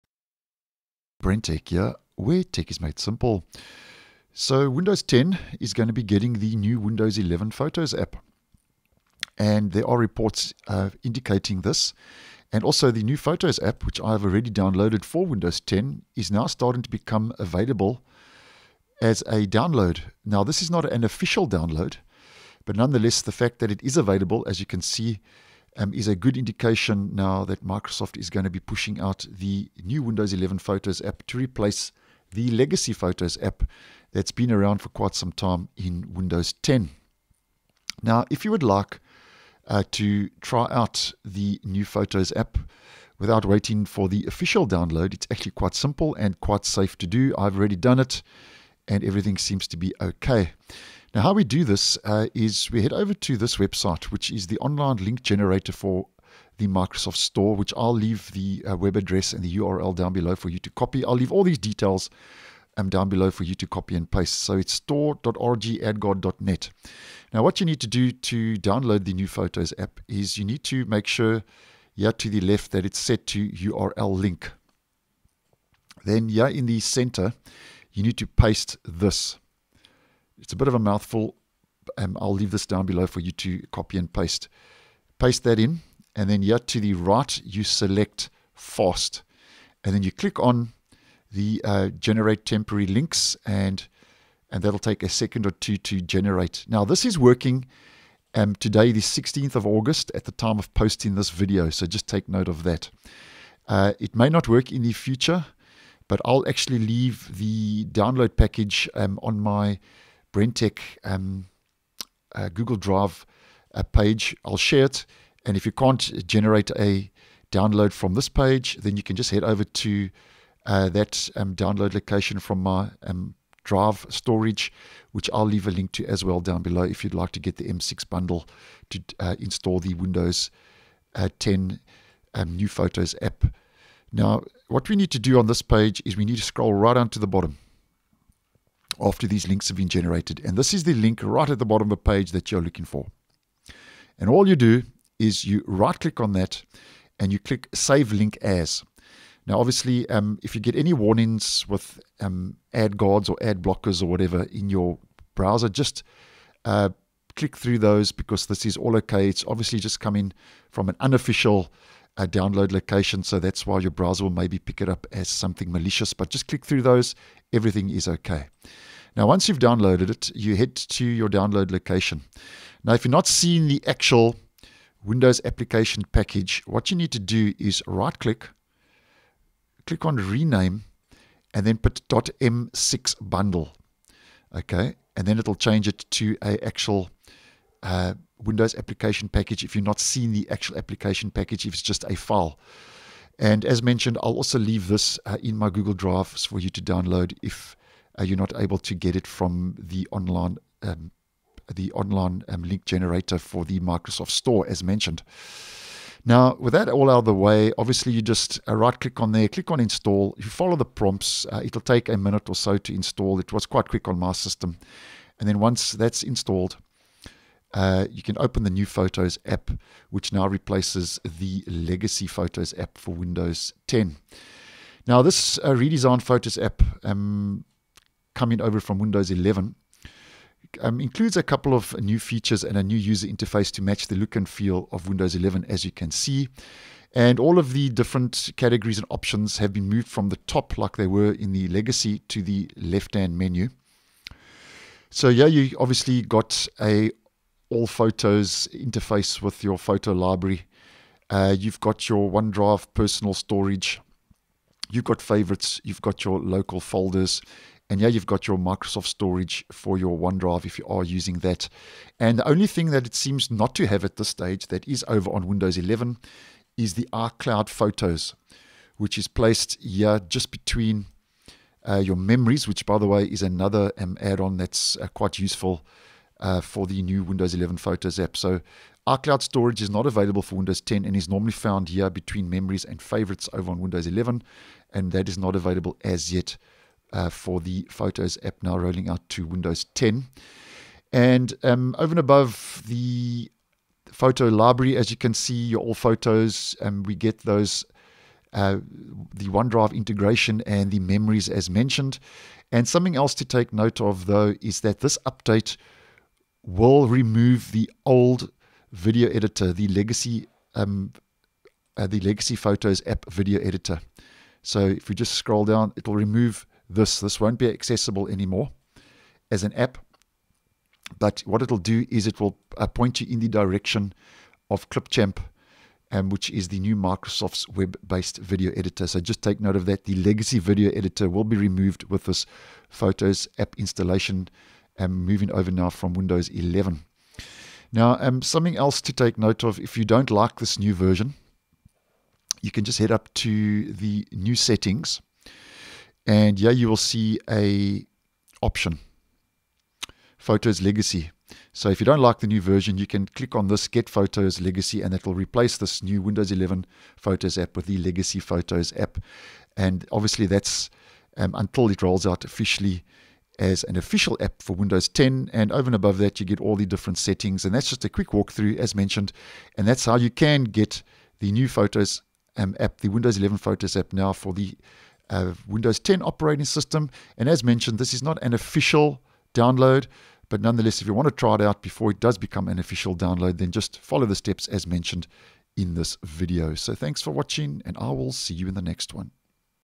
How to get the new Windows 11 Photos app on Windows 10 Now | Coming soon
As posted by Neowin the new update for the Photos app is coming soon to Windows 10, that includes a redesigned UI with photo library management improvements, better OneDrive integration, enhanced "Memories" and more. Here's how you can try the new Photos app now without waiting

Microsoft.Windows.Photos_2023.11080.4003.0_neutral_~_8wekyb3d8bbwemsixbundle